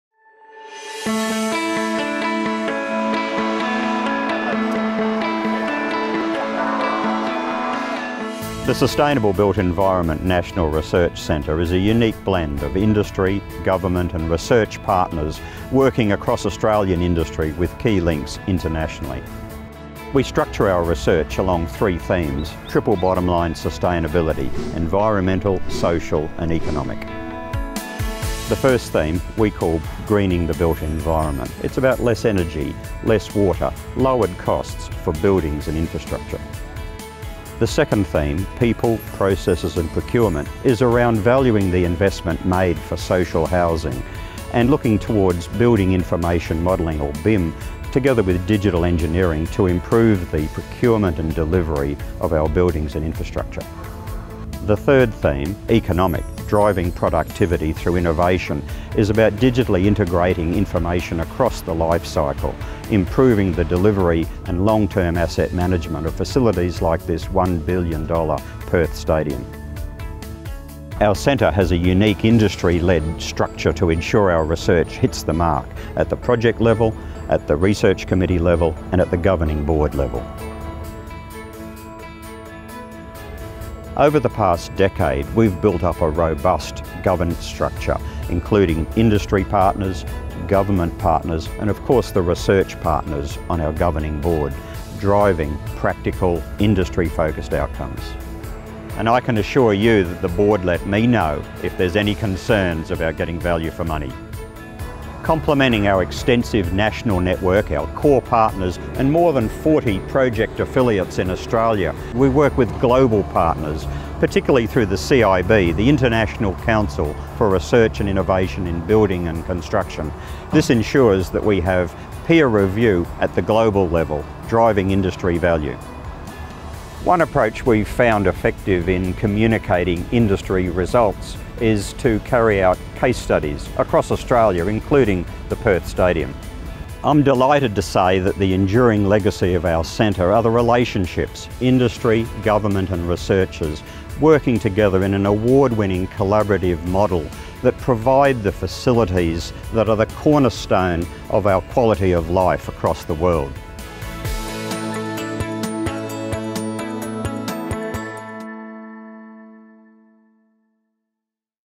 About the Australian Sustainable Built Environment National Research Centre (SBEnrc)
This video gives a brief introduction to the Sustainable Built Environment National Research Centre (SBEnrc) by Dr Keith Hampson, Chief Executive Officer.